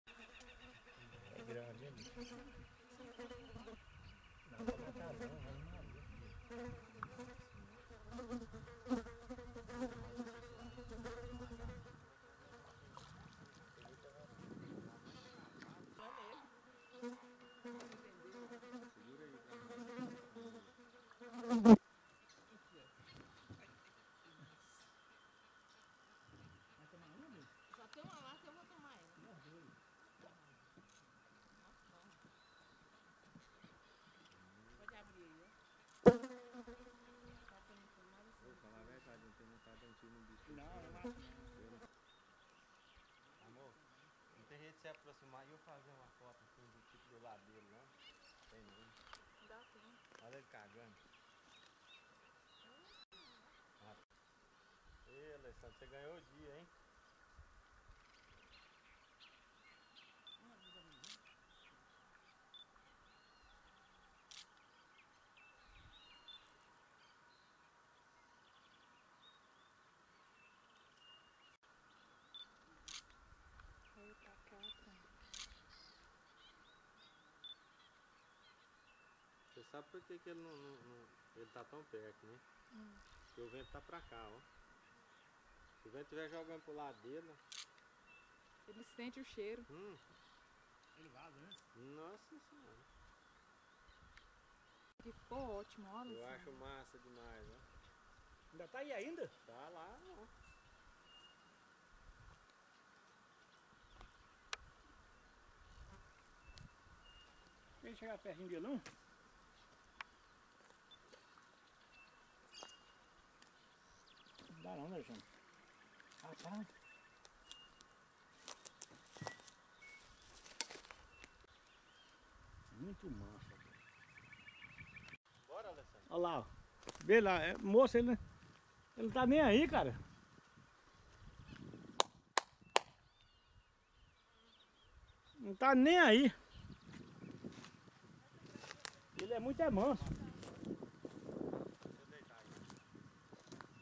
VEADO AVISTADO NO RIO ARAGUAIA
VEADO ENCONTRADO NA REGIÃO DE LUIS ALVES-RIO ARAGUAIA